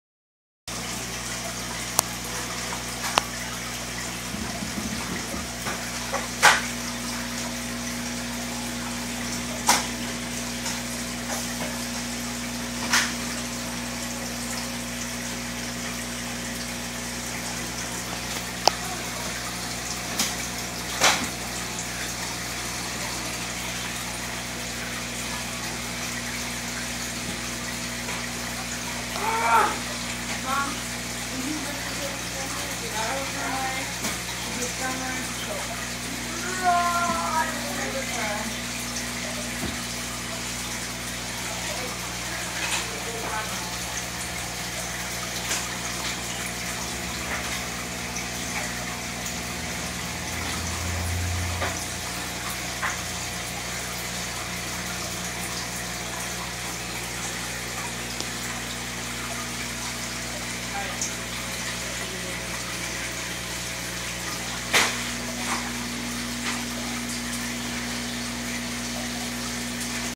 Baby wolves (dovii fry)
Just a quick update video of my dovii fry (wolf cichlids)